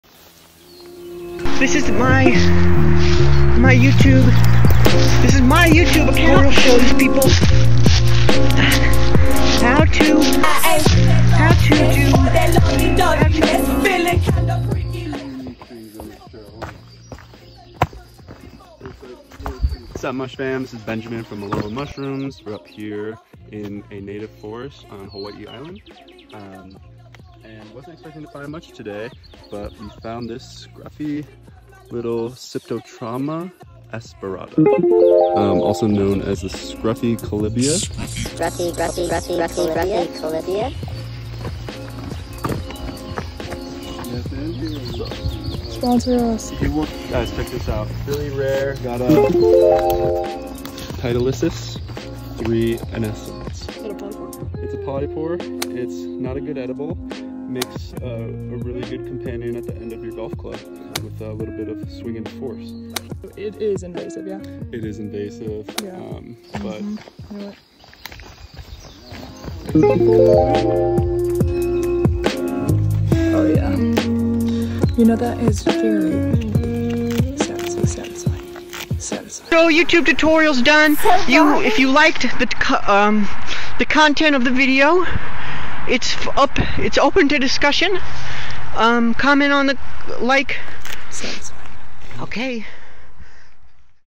Social Distance Foray! 🍄
Foraging is social distancing! 🌈✨ Foraging for wild mushrooms with your Quaran-team is a suuuuper neat way to socially distance & reconnect with good ol’ great outdoors. Fung times!